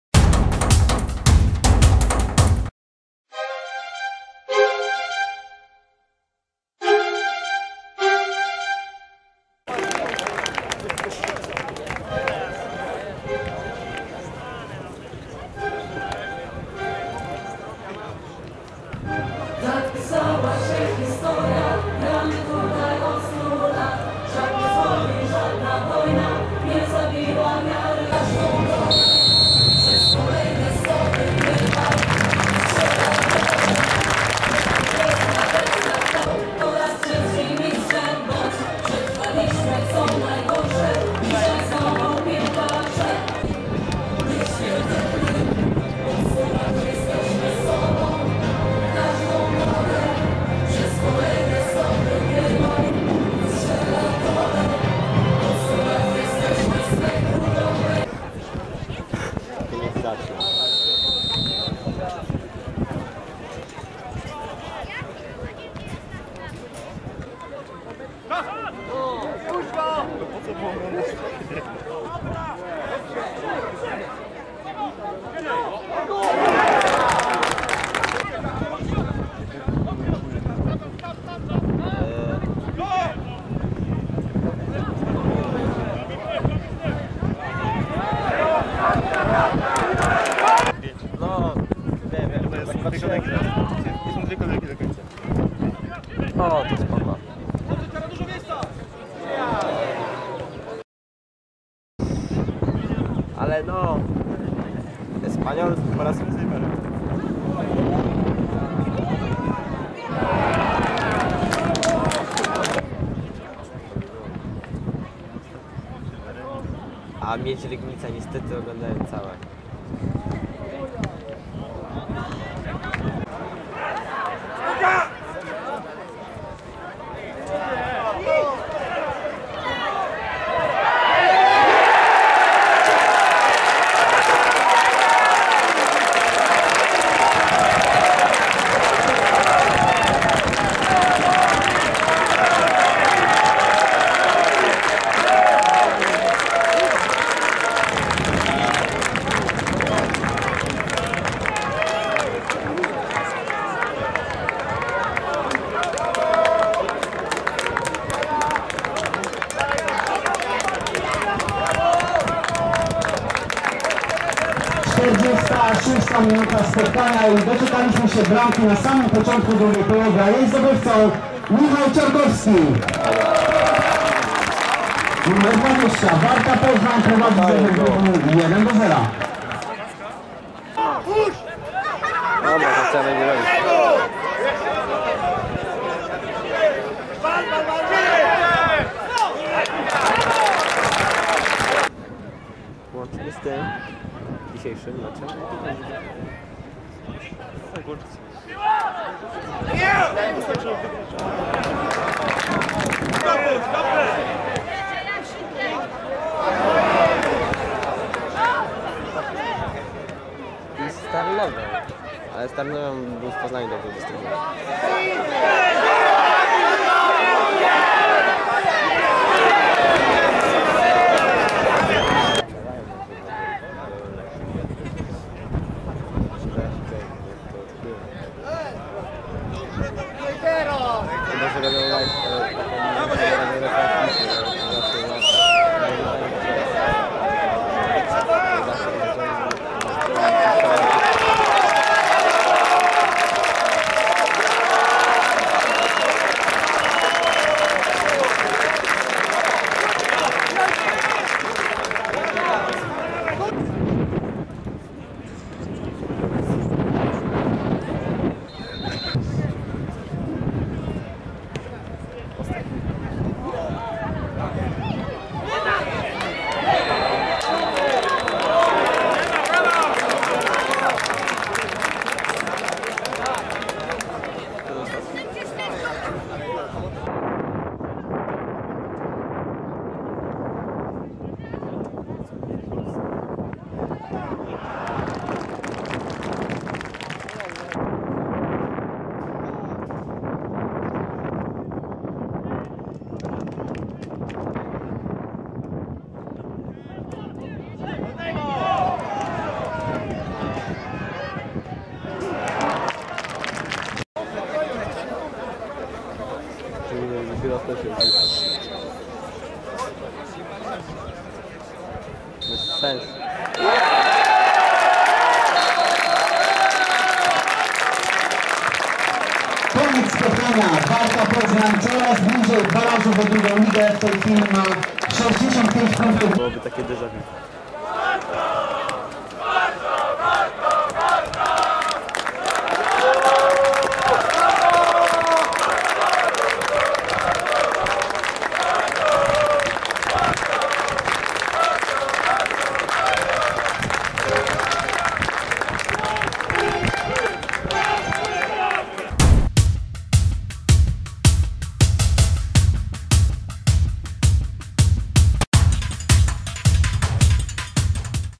Warta Poznań - Lech II Poznań / 16.05.2015 / Skrót
16 maja 2015 roku na Stadionie przy Drodze Dębińskiej miał miejsce mecz 29. kolejki III ligi, grupy kujawsko-pomorsko-wielkopolskiej, w którym Warta Poznań wygrała 1:0 z Lechem II Poznań.